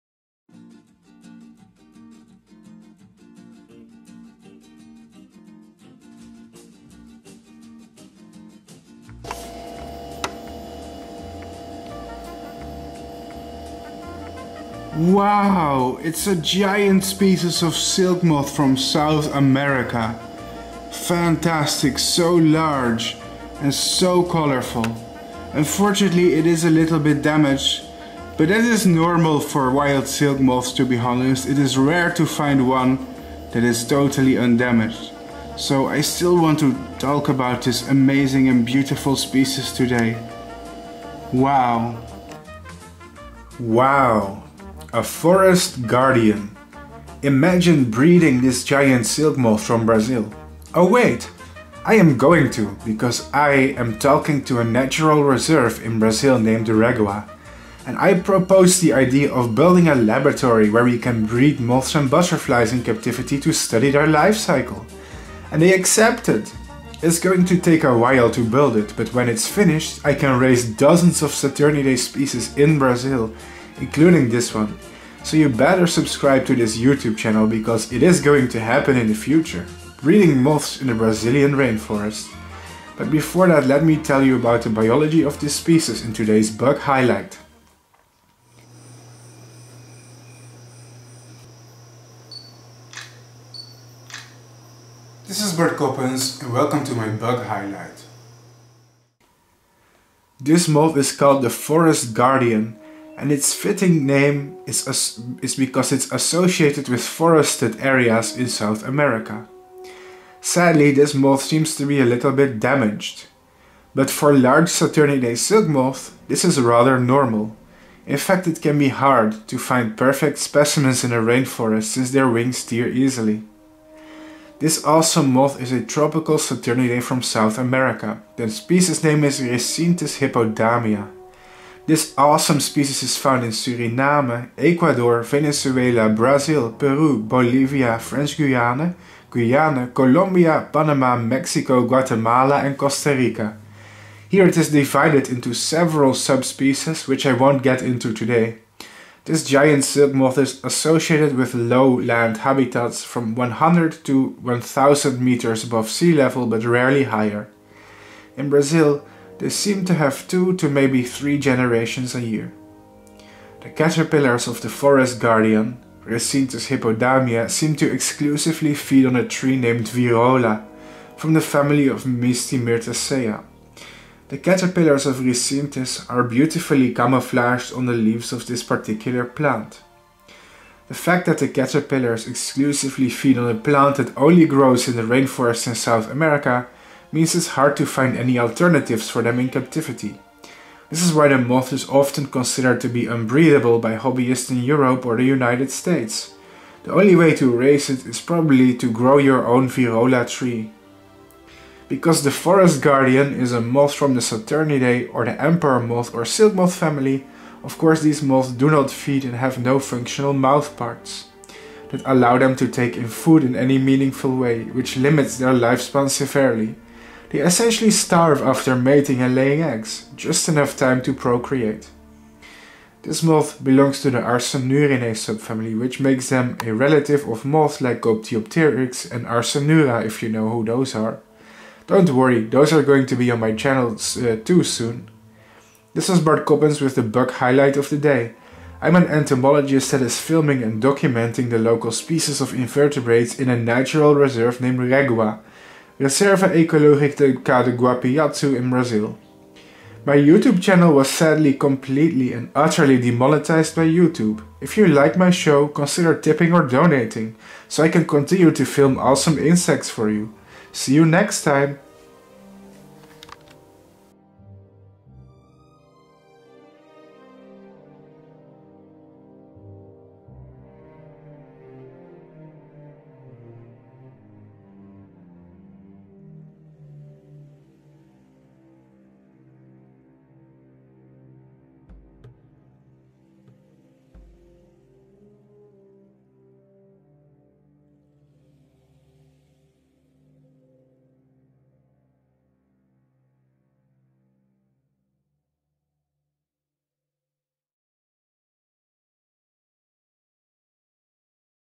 Forest Guardian: Rhescyntis hippodamia / GIANT MOTH from Brazil, REGUA - Bug Highlight
Rhescyntis hippodamia / Saturniidae / Forest Guardian / Brazil